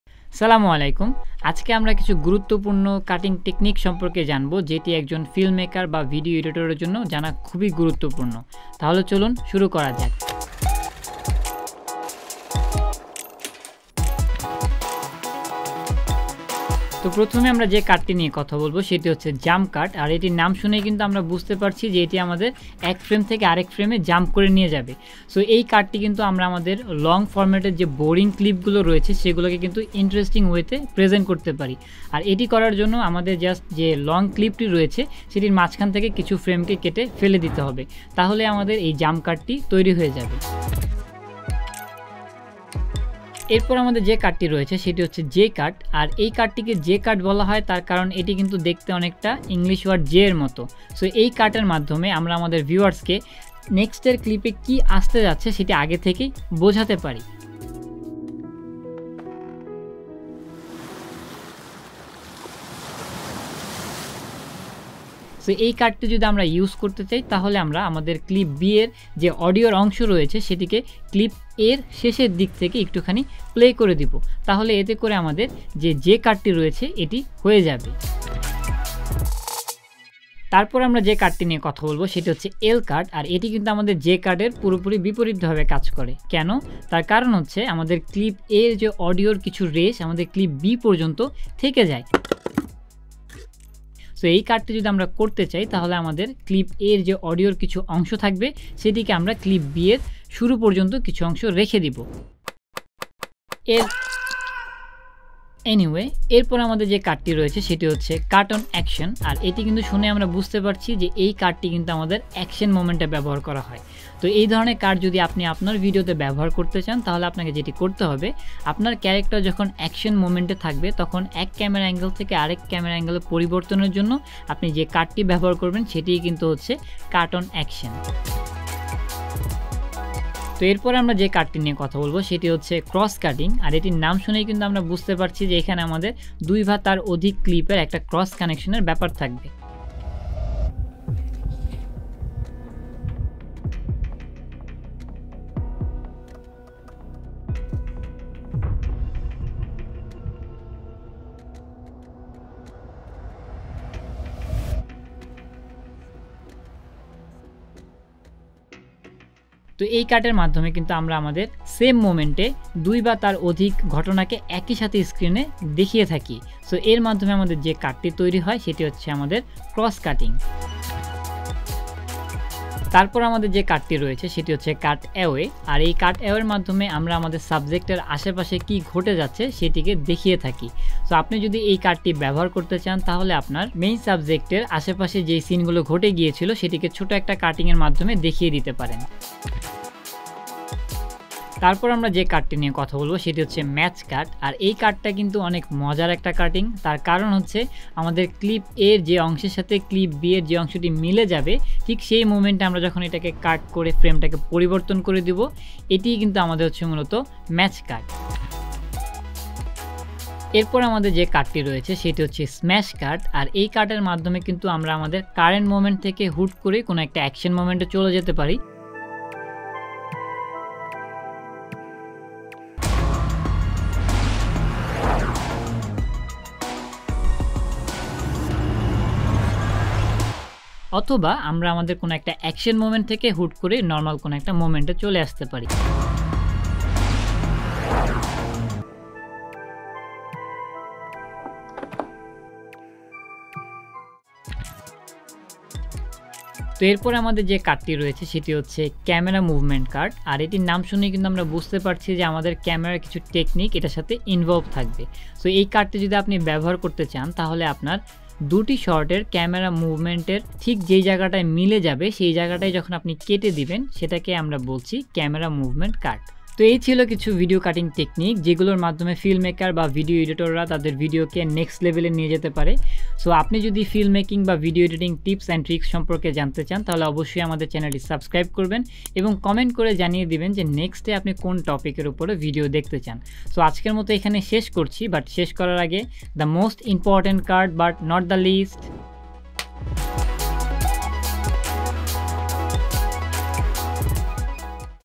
9 Video Cuts Every Video Editor Should Know | Video Editing Tips & Tricks
Video Editing এমন একটি সেক্টর , যেখানে সবসময়ই সৃজনশীলতার বিকাশ ঘটানো সম্ভব। প্রযুক্তির বিকাশের সঙ্গে সঙ্গে প্রতিনিয়তই বাড়ছে এ ক্ষেত্রে কাজের সুযোগ। তবে একজন Video Editor কে প্রচলিত বিষয়ের বাইরেও জানতে হবে অনেক কিছু। এমনি কিছু ইউনিক ও মজার ৯টি ভিডিও কাট সহজে শেখার উপায় জানতে পারবেন এই ভিডিও থেকে।

Chapters:
0:00 Intro
0:14 Jump Cut
0:41 J-Cut
1:18 L-Cut
1:45 Cut on Action
2:08 Cross Cutting
2:50 Cut Away
3:09 Match Cut
3:28 Smash Cut
4:04 Camera Movement Cut
4:26 Tips
4:45 The Most Important Cut

Stay always connected with our channel for something new every week to build yourself as an IT Professional.